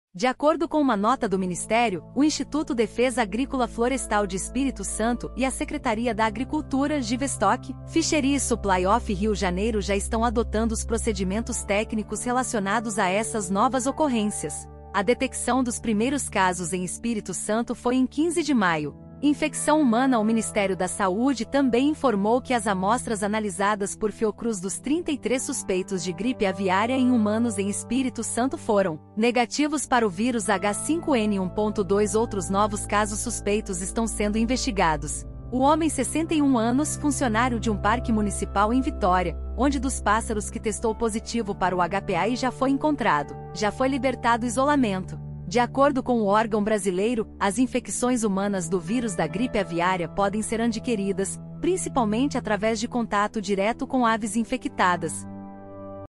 Brasil confirma mais dois novos casos de gripe aviária
De acordo com uma nota do Ministério, o Instituto Defesa Agrícola Florestal de Espírito Santo e a Secretaria da Agricultura, Givestock, Fisheries Supply of Rio Janeiro já estão adotando os procedimentos técnicos relacionados aessas novas ocorrências. A detecção dos primeiros casos em Espírito Santo foi em 15 de maio. infecção humana O Ministério da Saúde também informou que as amostras analisadas por Fiocruz dos 33 suspeitos de gripe aviária em humanos em Espírito Santo foram negativos para o vírus H5N1.Dois outros novos casos suspeitos estão sendo investigados. O homem 61 anos, funcionário de um parque municipal em Vitória, onde dos pássaros que testou positivo para o HPAI já foi encontrado, já foi libertado isolamento. De acordo com o órgão brasileiro, as infecções humanas do vírus da gripe aviária podem ser adquiridas, principalmente através de contato direto com aves infectadas .